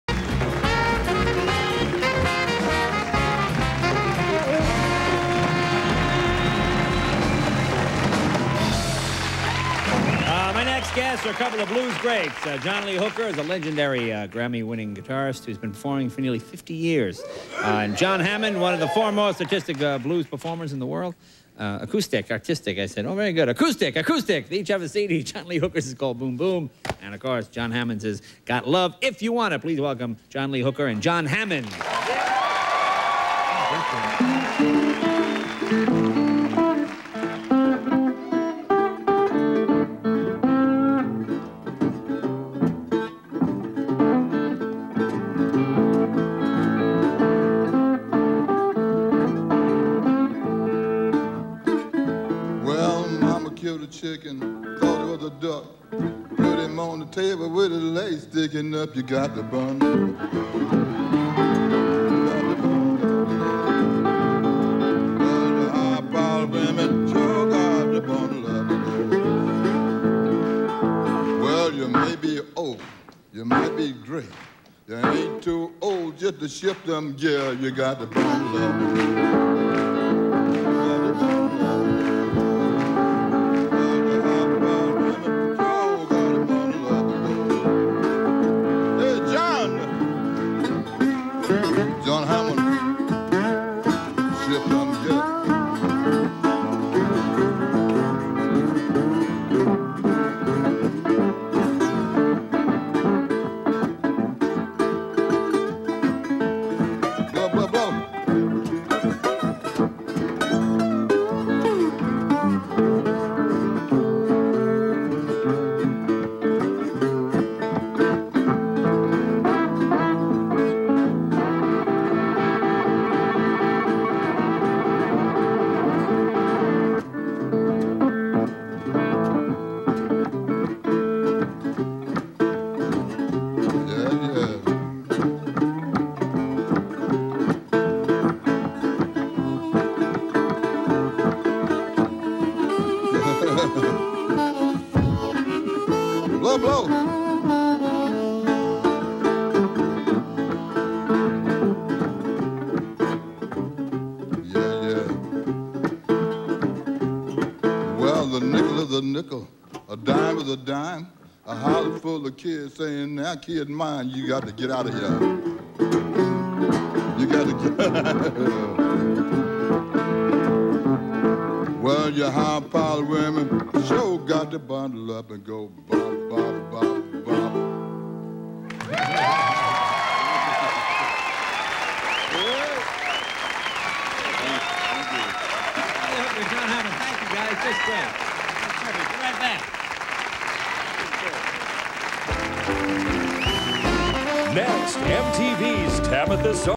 John Lee Hooker and John Hammond Jr [1992]
[Stereo]  Blues legend jams with younger generation blues guitarist, as a duo.